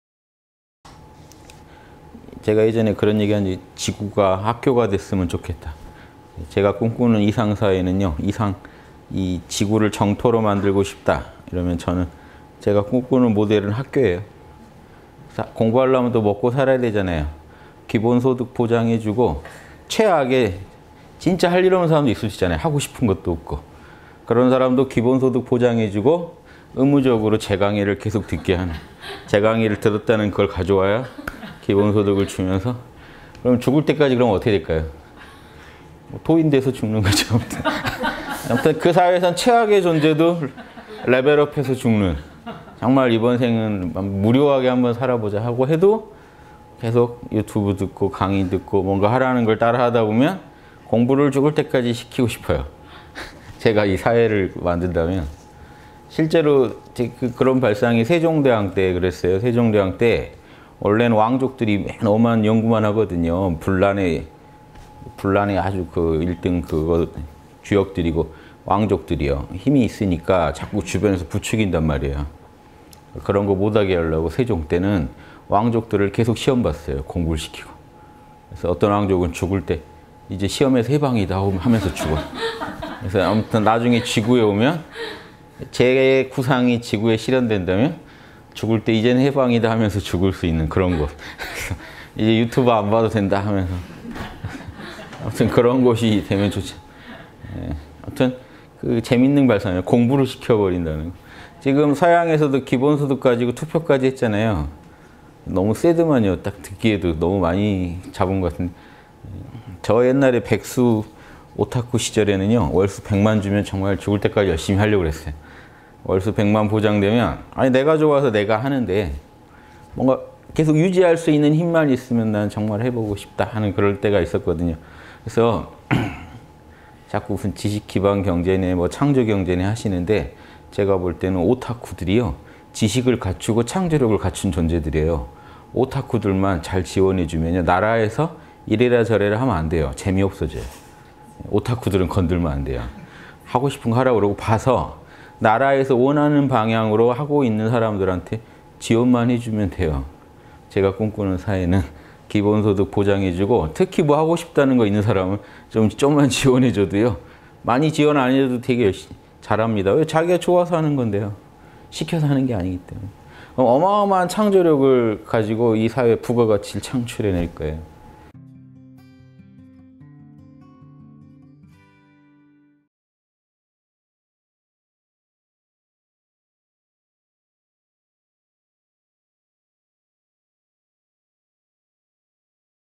[3분 양심] 학교사회 _홍익학당.윤홍식
(160707 목밤대담) 제가 예전에 그런 얘기 한 적 있는데,
지구가 학교가 됐으면 좋겠다.
제가 꿈꾸는 이상사회는요.
이상, 이 지구를 정토(淨土)로 만들고 싶다.
그러면 저는, 제가 꿈꾸는 모델은 학교에요.

그래서 공부하려면 또 먹고 살아야 하잖아요.
기본 소득 보장해주고
최악의, 진짜 할 일 없는 사람들 있을 수 있잖아요.
하고 싶은 것도 없고.
그런 사람도 기본소득 보장해주고,
의무적으로 제 강의를 계속 듣게 하는
제 강의를 들었다는 그걸 가져와야
기본소득을 주면서

그럼 죽을 때까지 그럼 어떻게 될까요?
도인 돼서 죽는 거죠. 아무튼
아무튼 그 사회에선 최악의 존재도 레벨업해서 죽는
정말 이번 생은 무료하게 한번 살아보자 하고 해도
계속 유튜브 듣고 강의 듣고
뭔가 하라고 하는걸 따라 하다 보면
공부를 죽을 때까지 시키고 싶어요.
제가 이 사회를 만든다면

실제로 그런 발상이 세종대왕 때 그랬어요.
세종대왕 때, 원래는 왕족들이
맨날, 어만 연구만 하거든요.
분란의, 분란의 아주 그 일등 주역들이고 왕족들이요.
힘이 있으니까 자꾸 주변에서 부추긴단 말이에요.

그런 거 못하게 하려고 세종 때는
왕족들을 계속 시험 봤어요. 공부를 시키고,
그래서 어떤 왕족은 죽을 때
이제 시험에서 해방이다 하고, 하면서 죽어요.

그래서 아무튼 나중에 지구에 오면
제 구상이 지구에서 실현된다면
죽을 때 이젠 해방이다. 하면서 죽을 수 있는 그런 곳
이제 유튜브 안봐도 된다 하면서
암튼 그런 곳이 되면 좋지 않을까
암튼 재밌는 발상이에요.
공부를 시켜버린다는 건

지금 서양에서도 기본 소득 가지고
투표까지 했잖아요. 너무 세더만요.
딱 듣기에도 너무 많이 잡은 거 같은데
저 옛날에 백수 오타쿠 시절에는요,
월수 100만 주면 정말 죽을 때까지
열심히 하려고 그랬어요.
월수 100만 보장되면,
아니 내가 좋아서 내가 하는데
뭔가 계속 유지할 수 있는 힘만 있으면
나는 정말 해보고 싶다.
그럴 때가 있었거든요.

그래서 자꾸 무슨 지식기반 경제네.
뭐 창조경제네 하시는데
제가 볼 때는 오타쿠들이요,
지식을 갖추고 창조력을 갖춘 존재들이에요.
오타쿠들만 잘 지원해주면요.
나라에서 이래라 저래라 하면 안 돼요. 재미없어져요.

오타쿠들은 건들면 안돼요.
하고 싶은 거 하라고 그러고, 봐서,
나라에서 원하는 방향으로 하고 있는 사람들한테
지원만 해주면 돼요.
제가 꿈꾸는 사회는 기본 소득 보장해주고,
특히 뭐 하고 싶다는 거 있는 사람은
조금만 지원해줘도요.
많이 지원 안 해도 되게 열심히 잘합니다.
왜 자기가 좋아서 하는 건데요.
시켜서 하는 게 아니기 때문에
그럼 어마어마한 창조력을 가지고
이사회의 부가가치를 창출해낼 거예요.